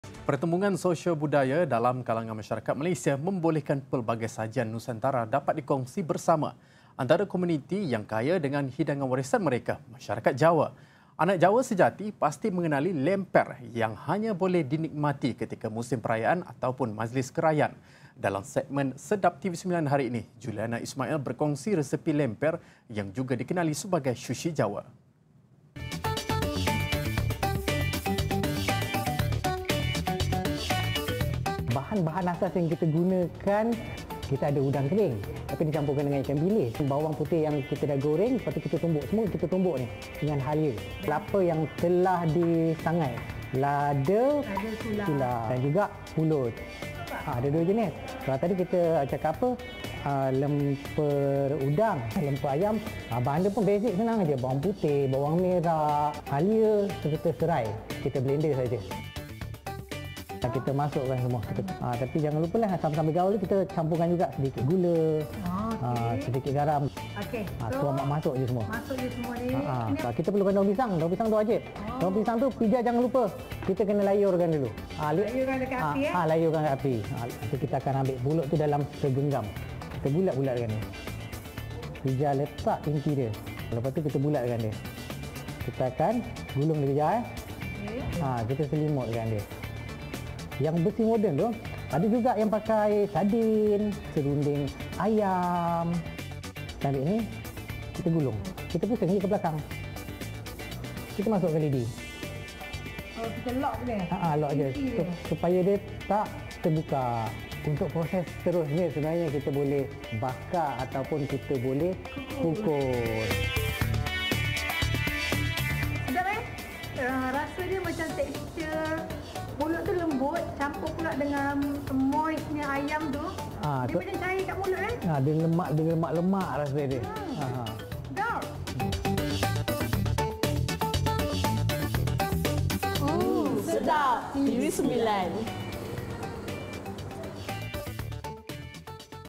Sedapnya Lemper Jawa
Anak Jawa sejati pasti mengenali Lemper yang hanya boleh dinikmati ketika musim perayaan ataupun majlis keraian.

Dalam segmen Sedap TV9 hari ini, Juliana Ismail berkongsi resepi Lemper yang juga dikenali sebagai sushi Jawa.